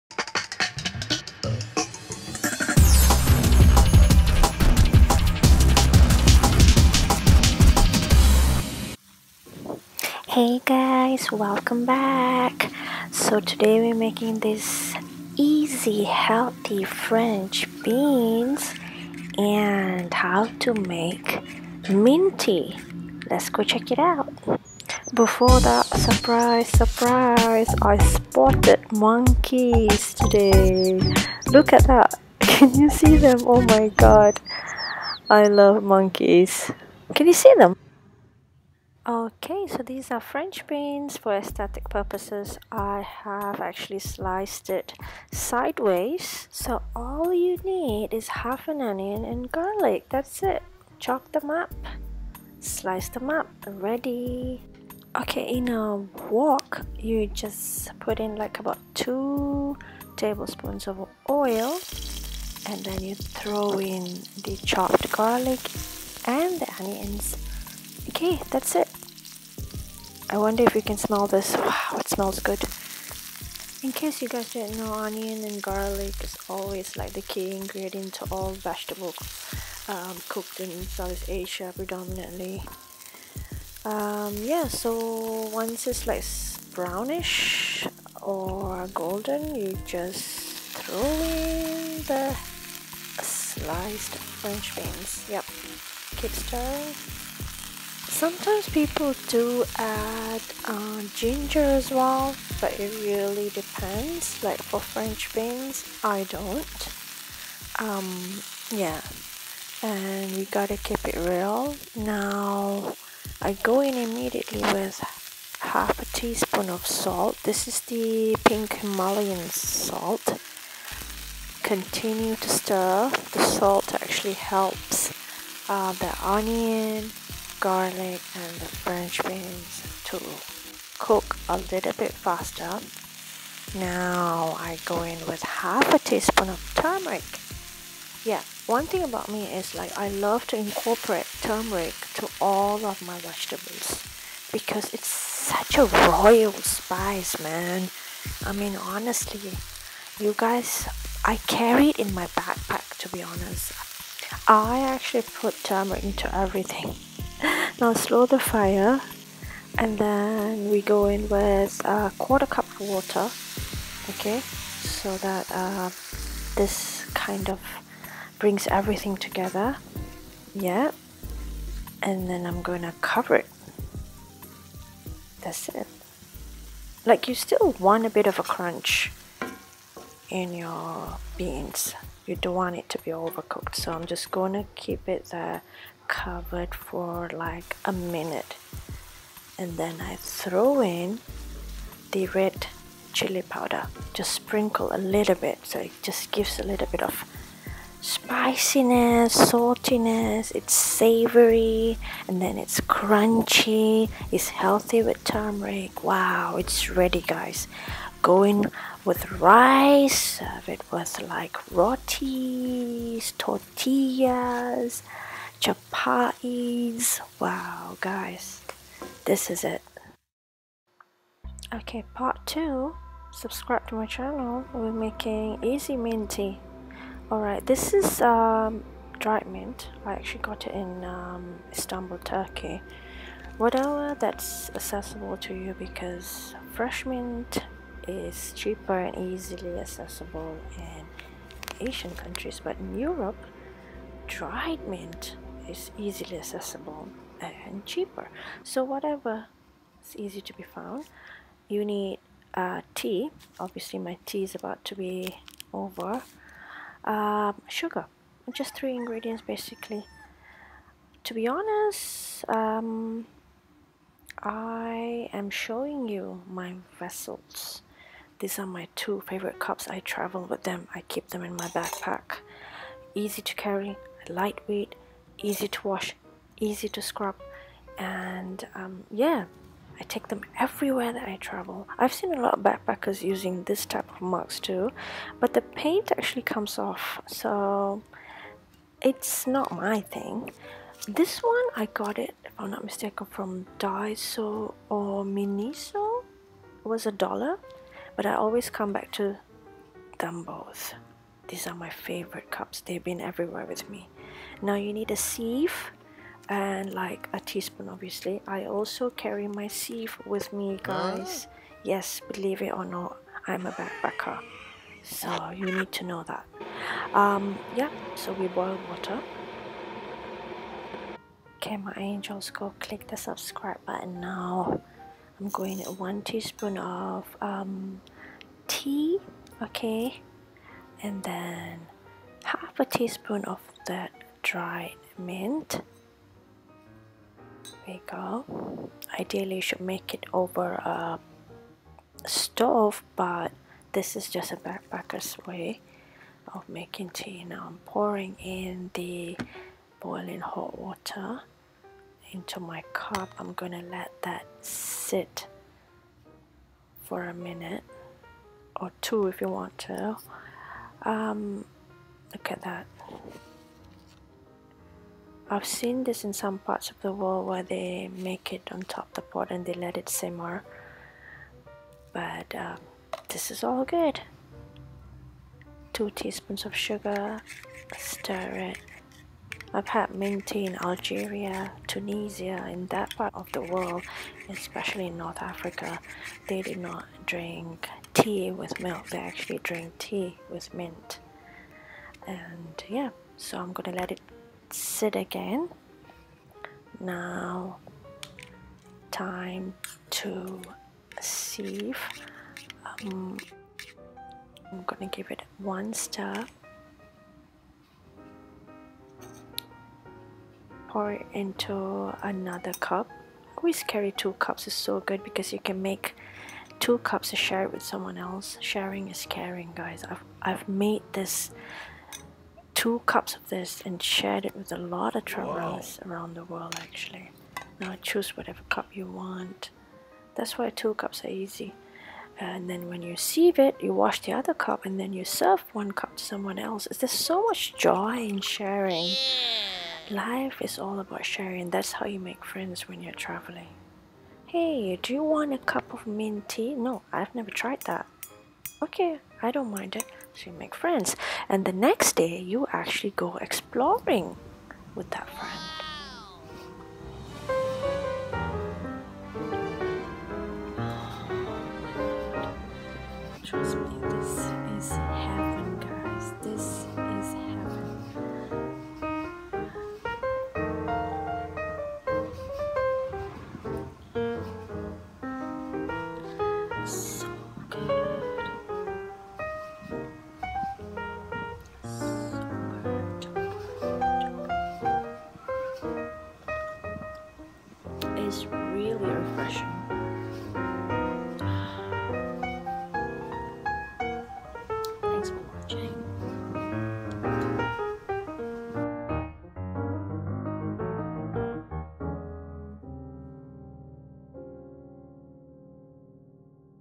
TRAVEL & COOK: YUMMY FRENCH BEANS (HOW TO MAKE MINT TEA) 🙌easyrecipe
Hello My Angels! In this video its a bit about travel and cooking. Yes here I share a recipe of how to make this delicious French Beans. It is so healthy and fresh plus vegan friendly. Definitely when we are on the road or living a nomadic lifestyle ...we enjoy a lot of food from outside. At times we do need our vegetable input into our system. This dish is so healthy that you can also serve it to your parent and children.
On the second part, I will show you the easiest way to make Mint Tea. This Tea is famous in many Arabic restaurants. However its widely made in North Africa at home and cafes. I had some of the most delicious Mint Tea in Algeria and Tunisia. Here is the "traveller's stye" of making this refreshing Tea.

✏️French Beans contains B9, K Manganese and C
✏️Also contains Thiamin, Folate, Riboflavin, Potassium and Magnesium